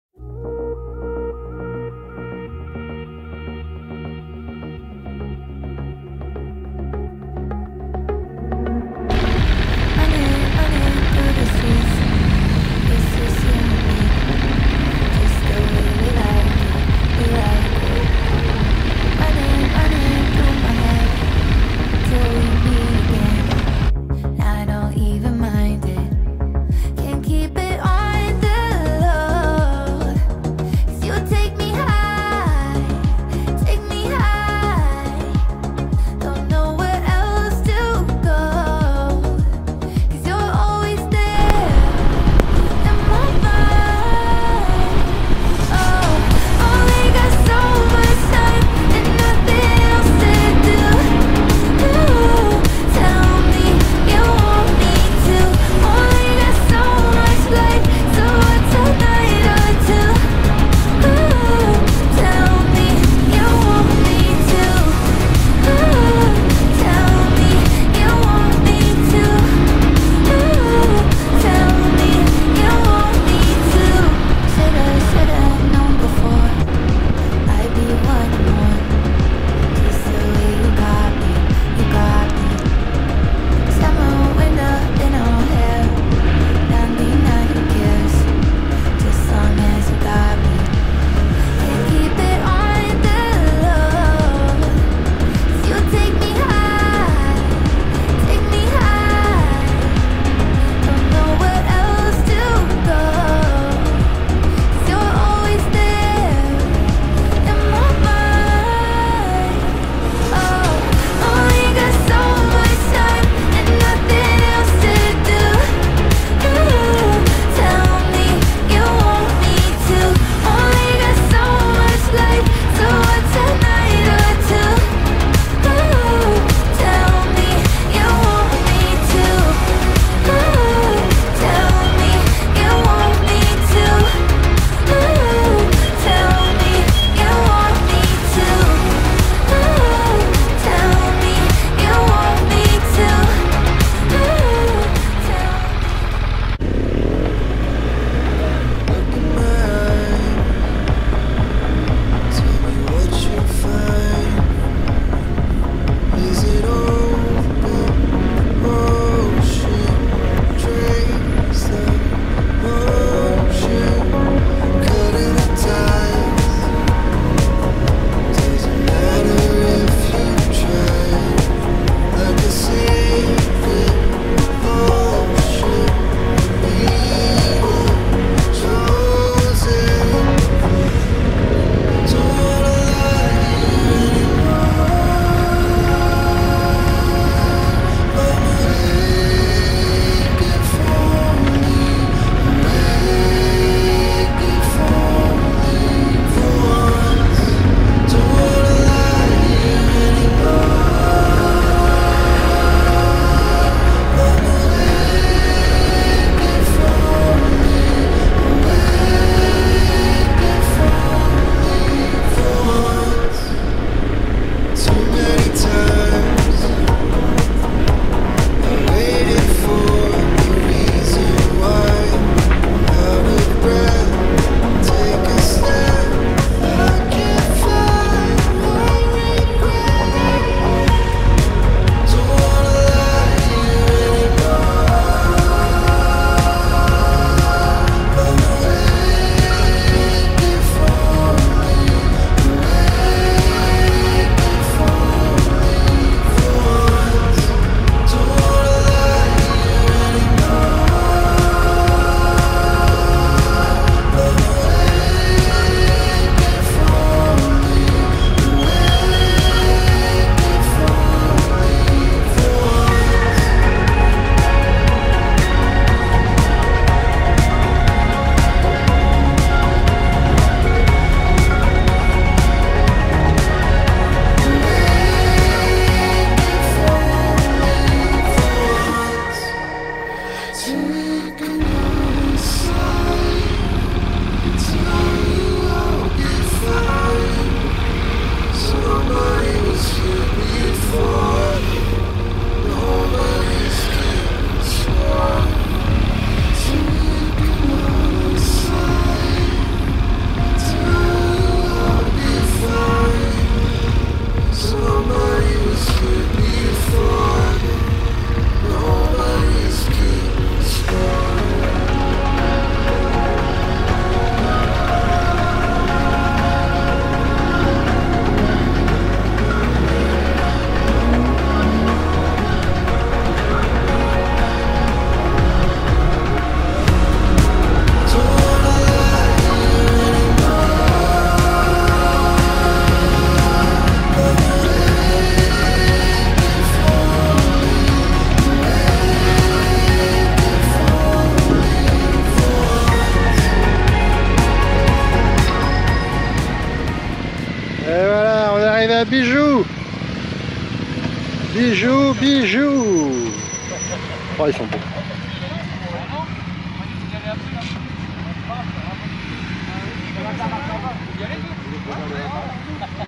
Trails du 17/ Direction le Limousin
Départ de Tonnat Charente ( 17 ) direction Saint Yrieix la Perche dans le Limousin, avec notre groupe de motards pour aller chercher des madeleines Bijou ou plutôt passer une super journée sur les routes à virolos, top !